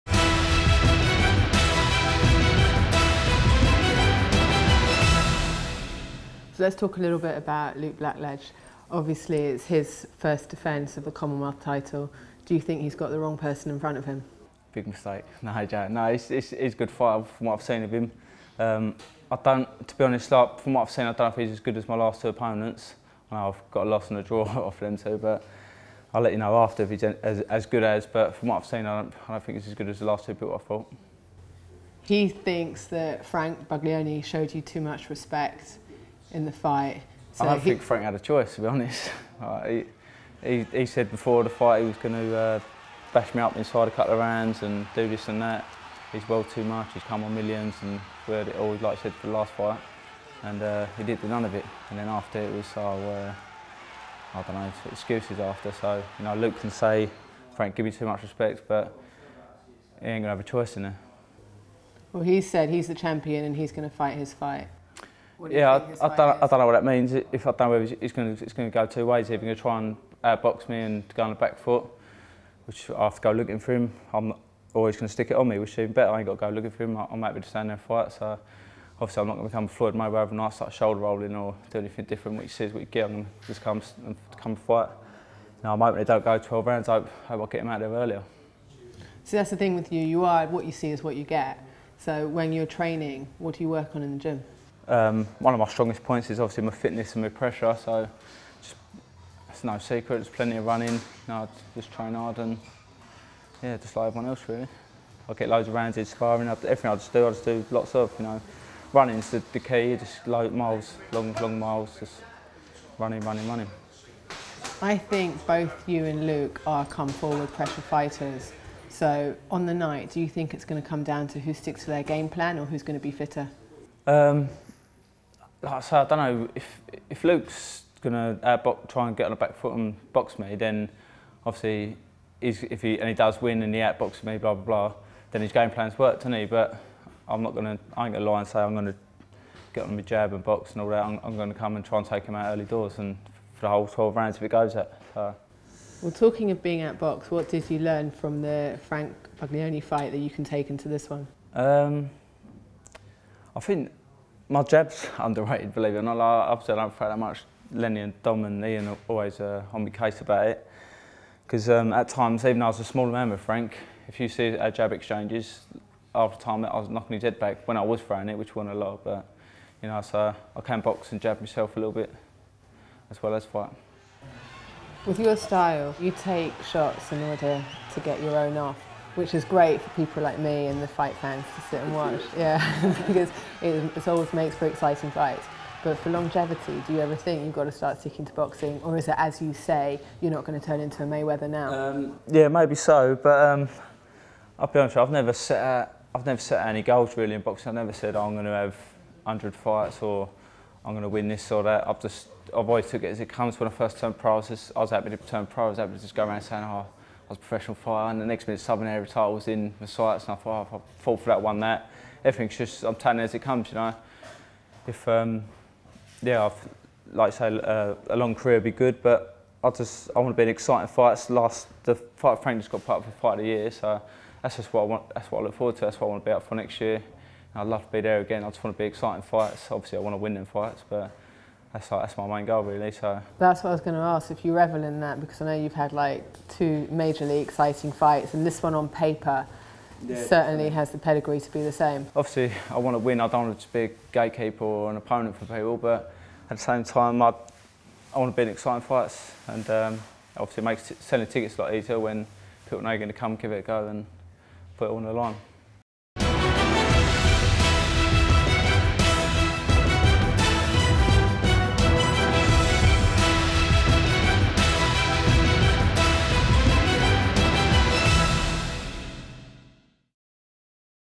"He's not as good as my last two opponents" Lee Markham on Luke Blackledge
Lee Markham discusses his fight against Luke Blackledge for the Commonwealth Super-Middleweight title on Saturday 10 October LIVE on BoxNation.